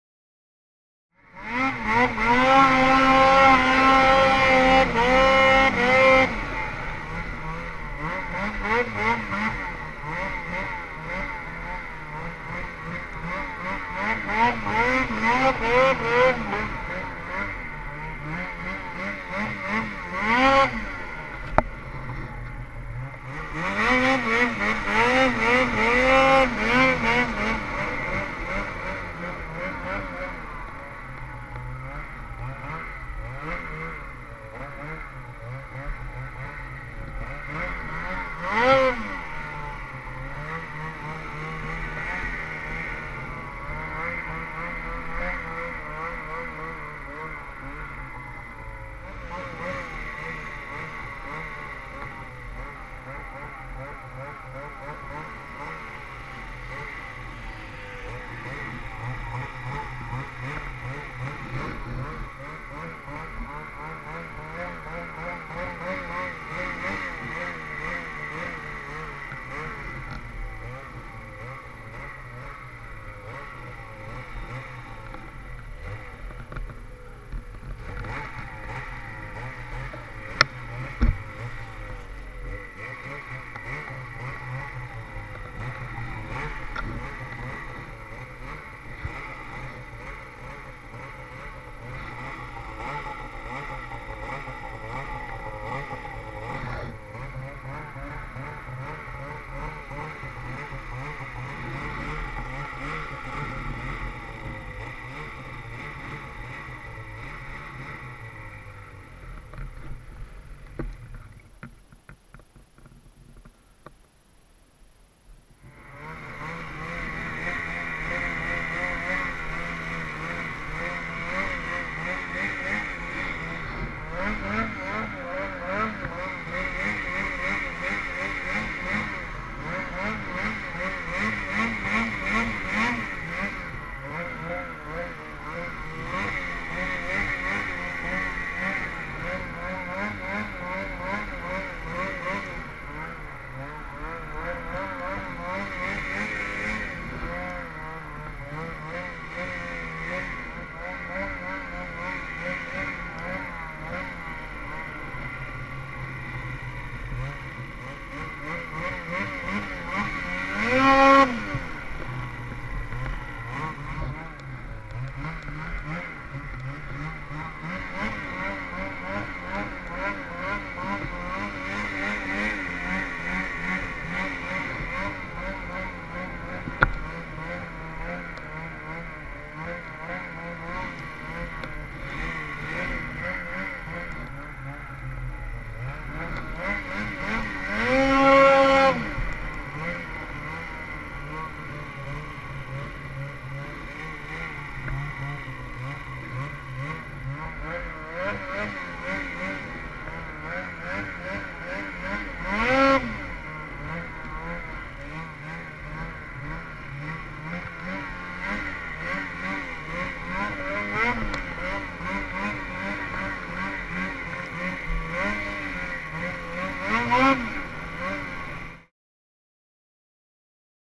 Day 3 Henning
A nice bluebird Day on Henning with David Scott and Michael, the camera angles are a bit off but I will have it fixed for the next videos thanks for watching subscribe to my channel for more content Thanks @skidoo_stu